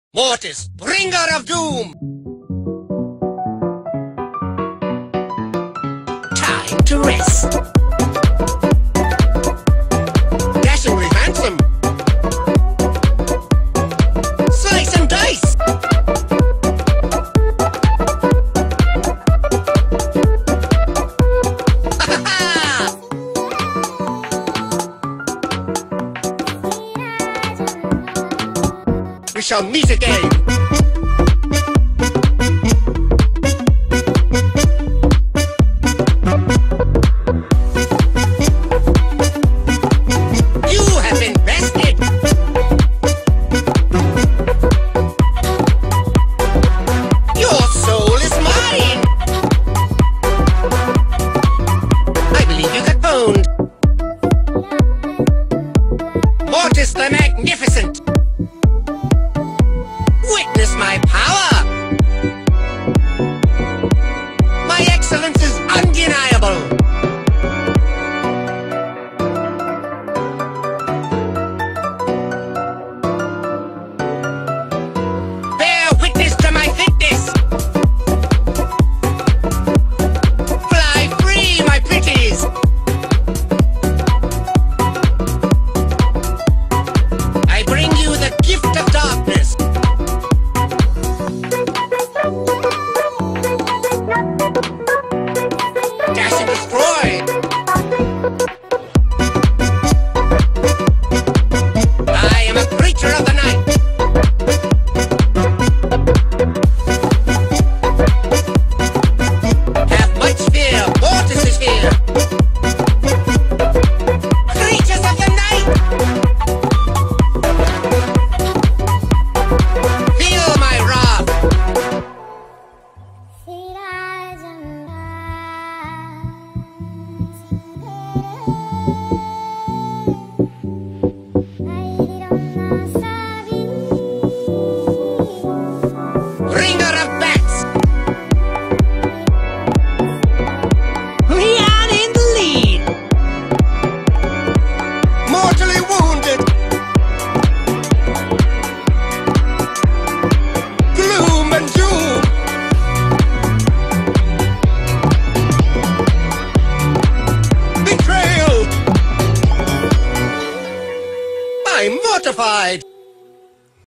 도적모티스를 어몽어스로 그리기_Drawing brawlstars_ Drawing AMONG US_ 브롤스타즈그리기 MEME among us #brawlstarsart
도적모티스를 어몽어스 버전으로 그렸습니다.
I drew the AMONG US version of the Brawlstars Rogue Mortis.
how to draw rogue mortis brawlstars